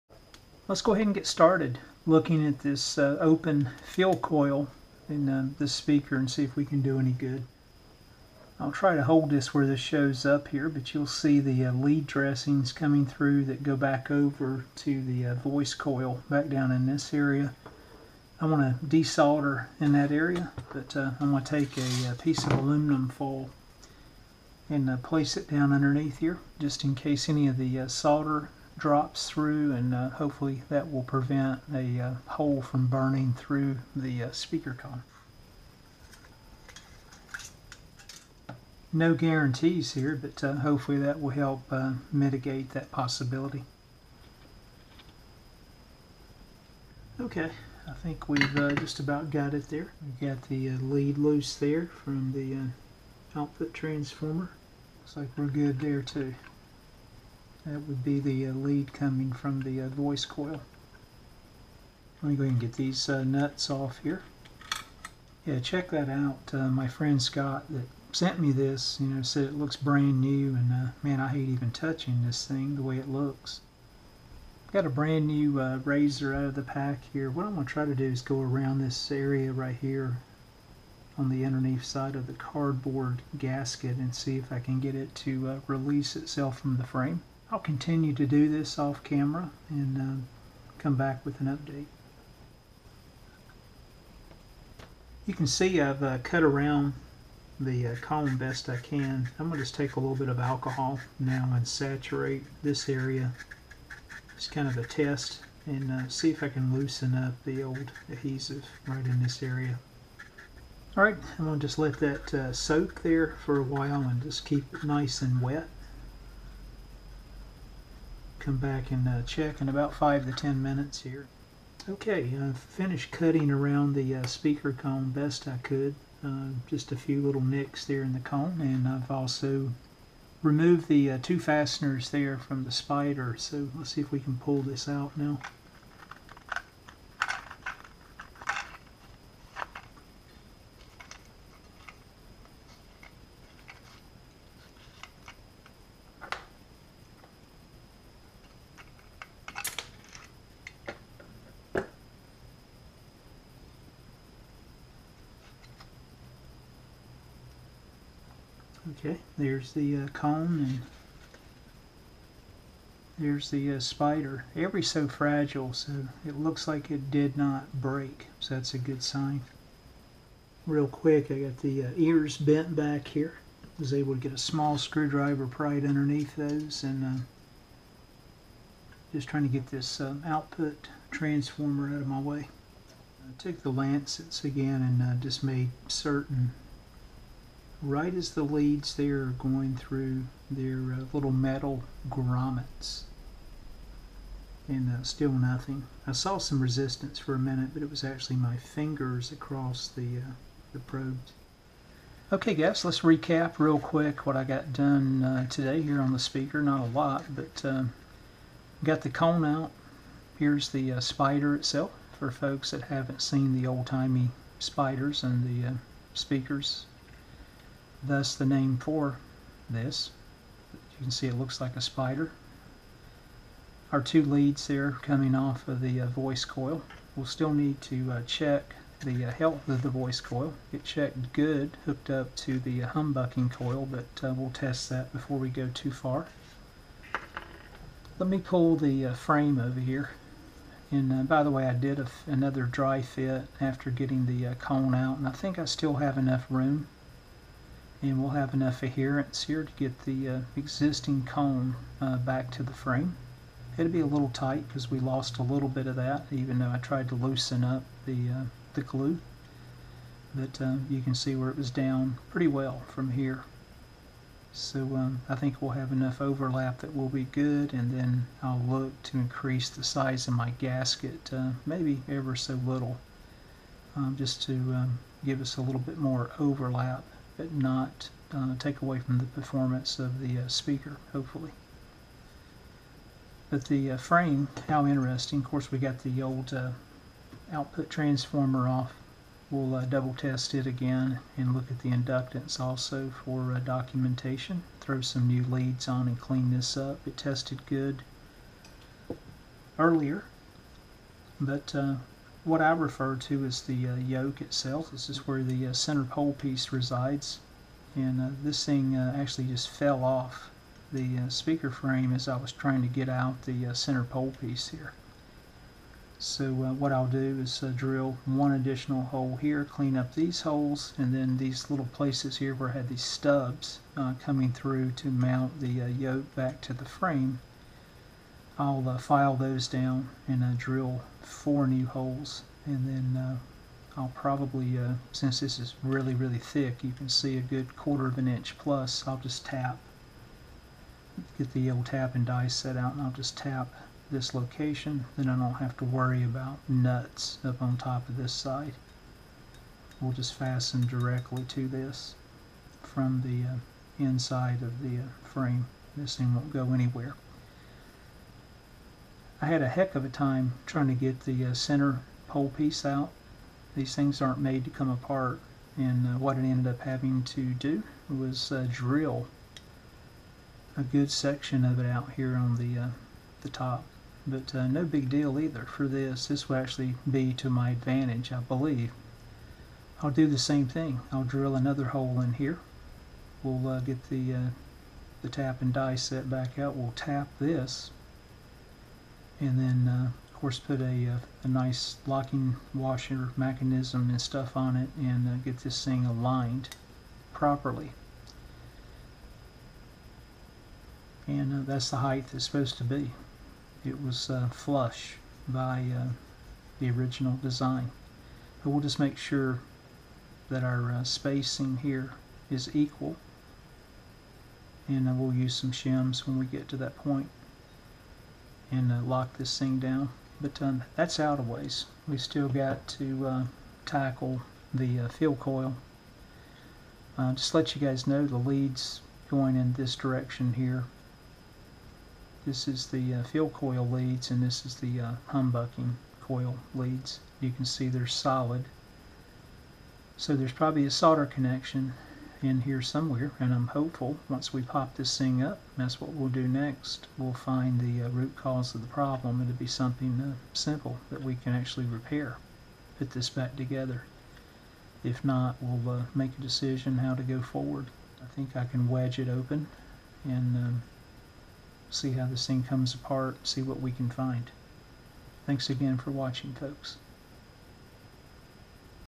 Dismantling The Speaker
Removing the speaker cone, spider, voice coil, output transformer and field coil from the speaker frame so additional testing can be performed on the open Field Coil.